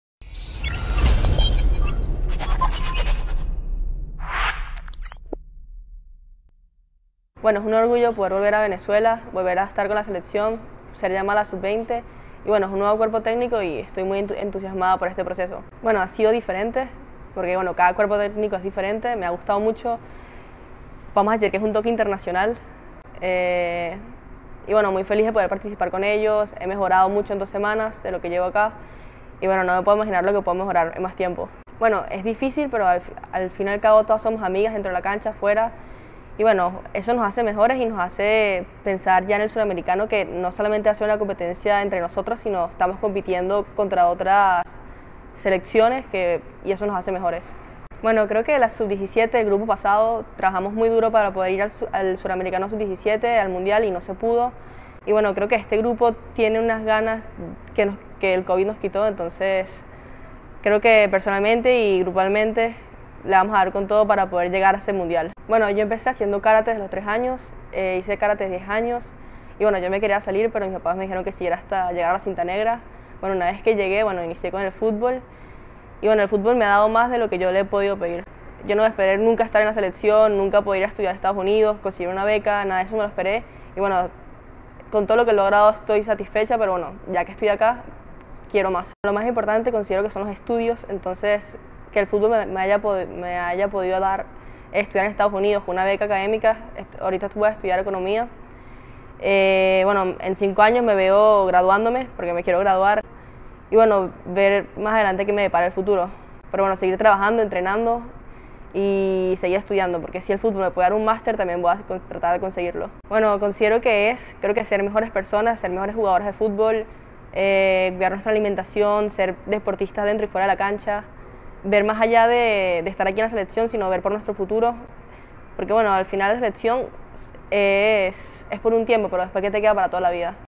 Femenino sub-20 | Entrevista a Bárbara Russo - Módulo 4
La defensora Bárbara Russo, oriunda de Caracas, cuenta parte de su historia como futbolista y además, cómo se prepara para el venidero Torneo CONMEBOL sub-20 femenino de Chile.